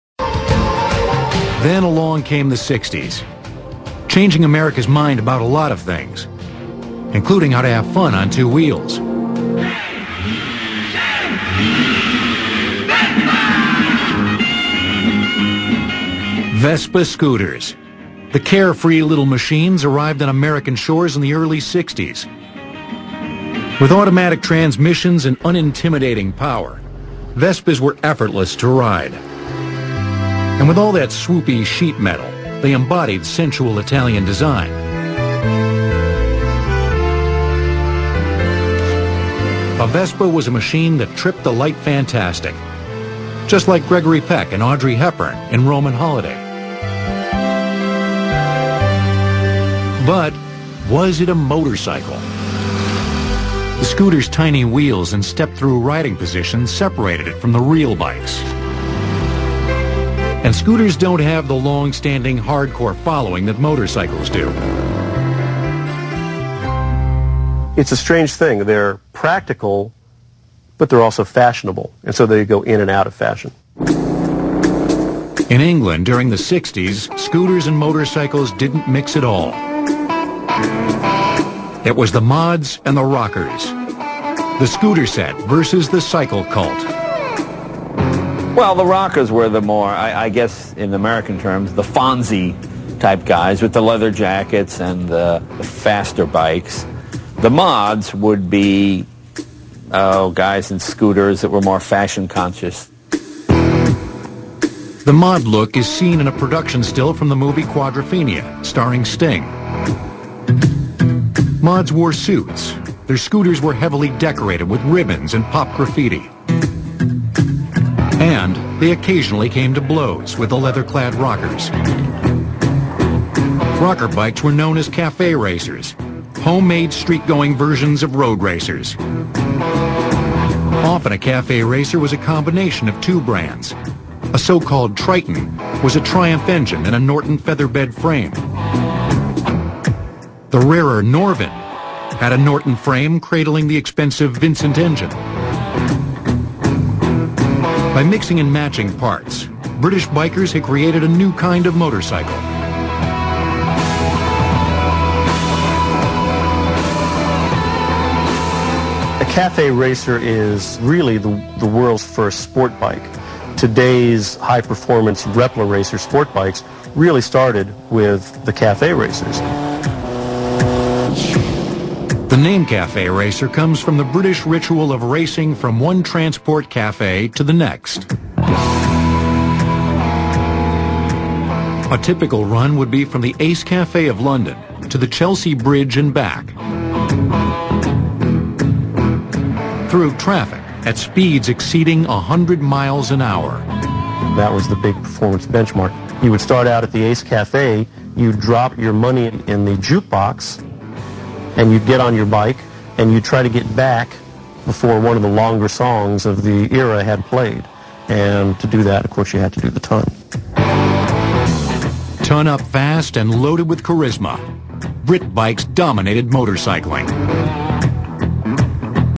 Scooters Versus Motorcycles History
This is a video about the history of scooters such as Vespas and motorcycles particularly cafe racers of the past like Triumphs and Nortons.